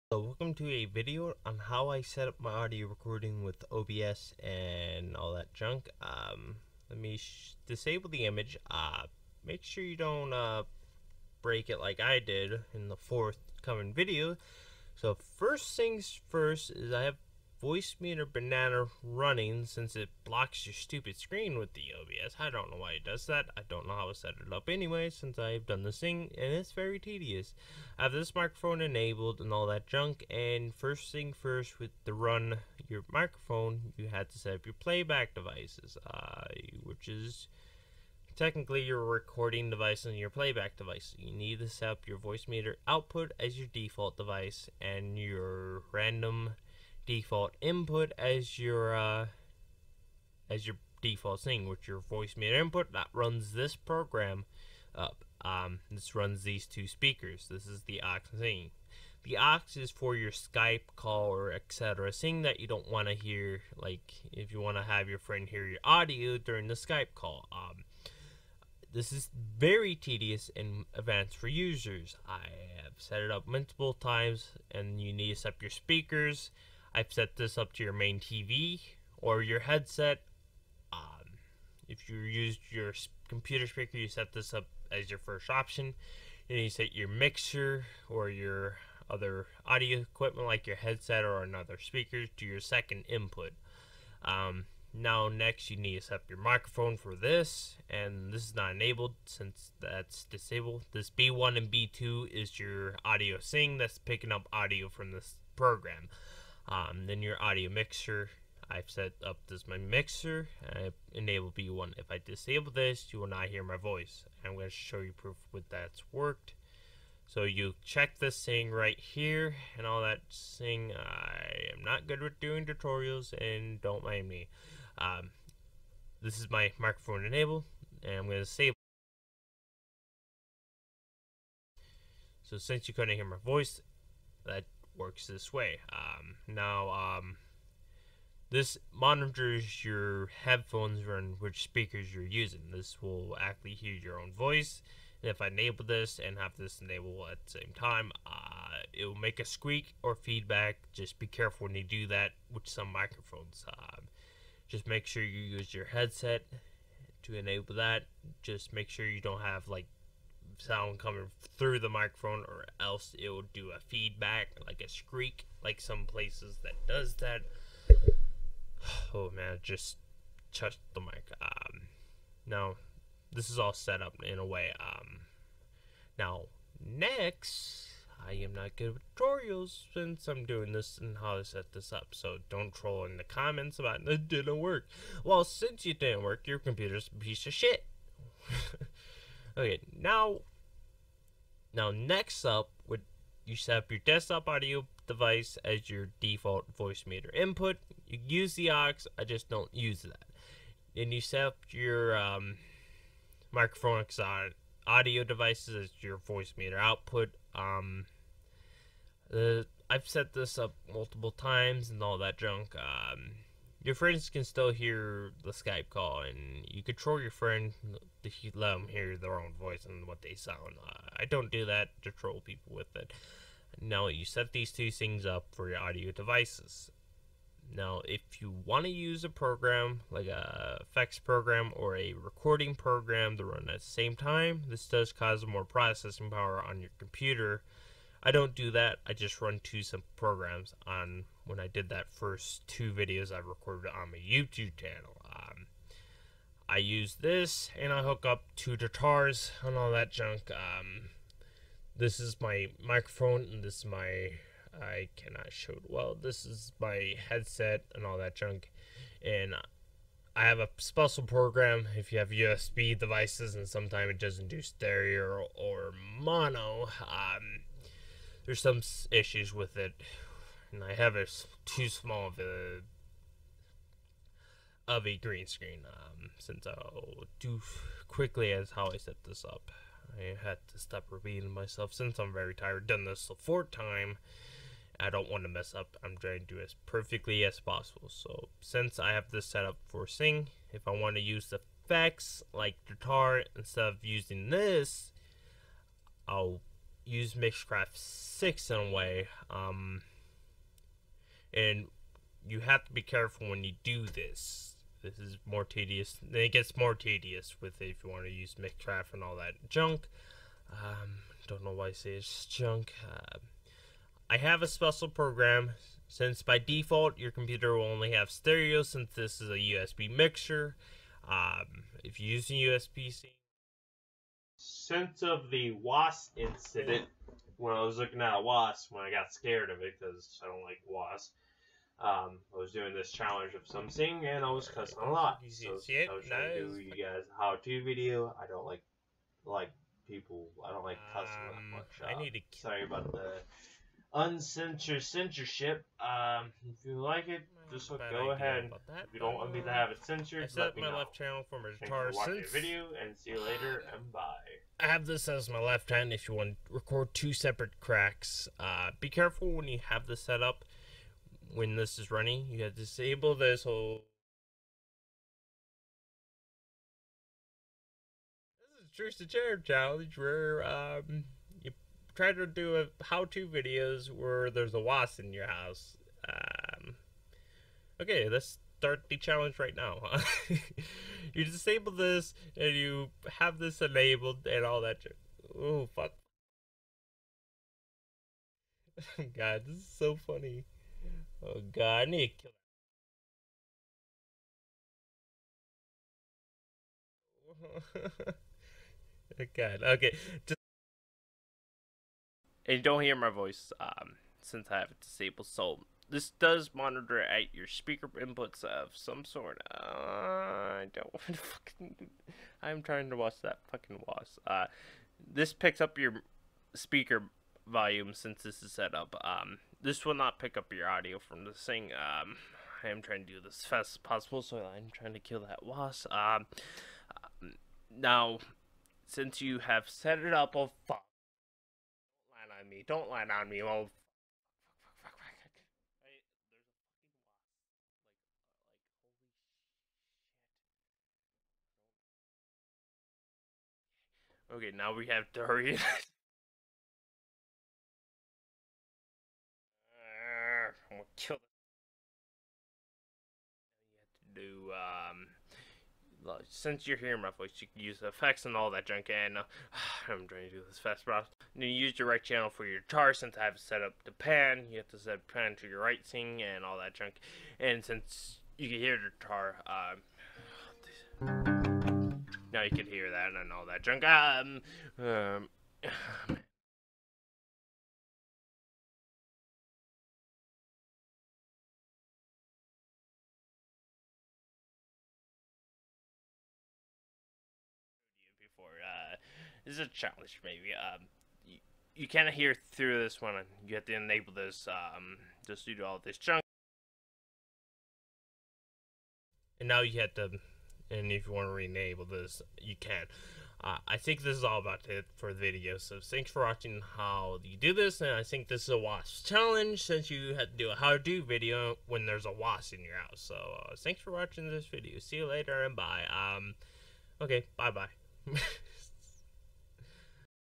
how to use Voice Meeter Banana for Mixcraft and OBS
Well this is short of a how to video on how to use Voice Meeter Banana for Mixcraft 6 and OBS at the same. These are my setting for how i setup for my music video. On my channel. So sorry that I don't make a a lot of since in the end of the video. So please don't say it didn't work.